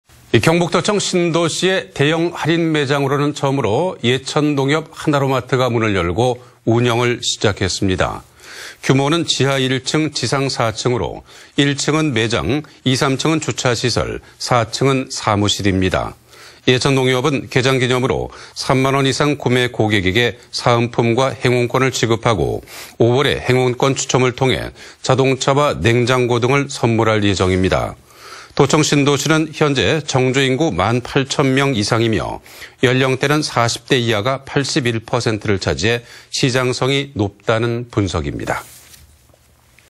경북도청 신도시에 대형 매장 입점/ 안동MBC
2019/02/26 10:53:45    작성자 : 이정희

경북도청 신도시에 대형 할인매장으로는
처음으로 예천농협 하나로마트가 문을 열고
운영을 시작했습니다.
◀ＥＮＤ▶

규모는 지하 1층 지상 4층으로 1층은 매장,
2~3층은 주차시설, 4층은 사무실입니다.

예천농협은 개장 기념으로 3만 원 이상 구매
고객에게 사은품과 행운권을 지급하고
5월 행운권 추첨을 통해 자동차와 냉장고 등을 선물할 예정입니다.

도청 신도시는 현재 정주 인구 만 8천 명
이상이고 연령대는 40대 이하가 81%를 차지해
시장성이 높다는 분석입니다.